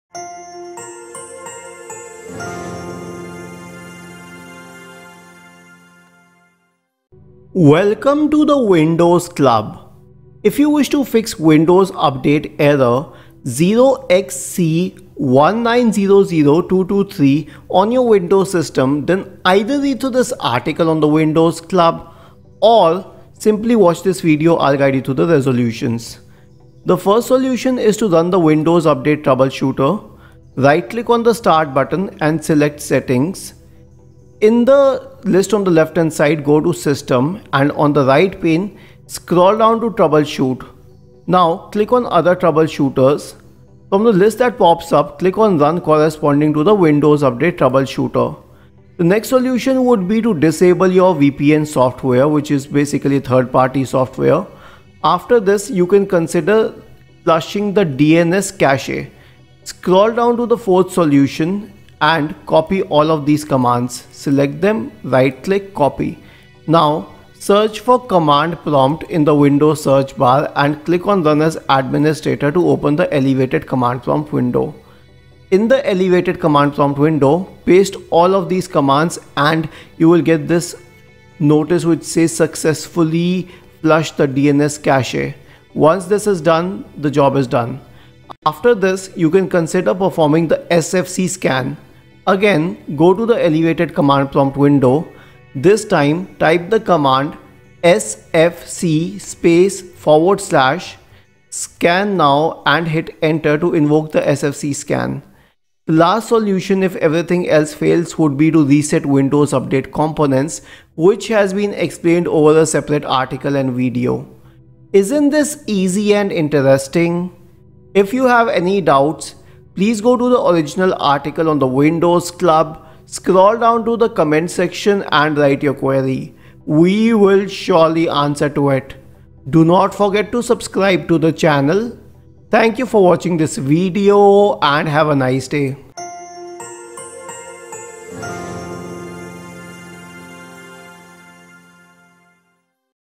How to fix Windows Update Error 0xc1900223
Many users are reporting a Windows Update error 0xc1900223 while trying to installing Windows Update. This problem especially comes when installing a Feature Update. With this error code, you could see an error message that states:

There were problems installing some updates, but we will try again later. If you keep seeing this and want to search the web or contact support for information, this may help:

Feature update to Windows 10 – Error 0xc1900223.

Here are a few fixes and suggestions:

1] Run Windows Update Troubleshooter

2] Run Media Creation Tool

3] Disable your VPN software

4] Flush the DNS cache

5] Perform DISM and SFC tools

6] Reset Windows update component

Timecodes:
0:00 Intro
0:27 Run Windows Update troubleshooter
0:50 Disable VPN
0:57 Flush DNS cache
1:28 SFC Scan
1:47 Reset Windows Updates Components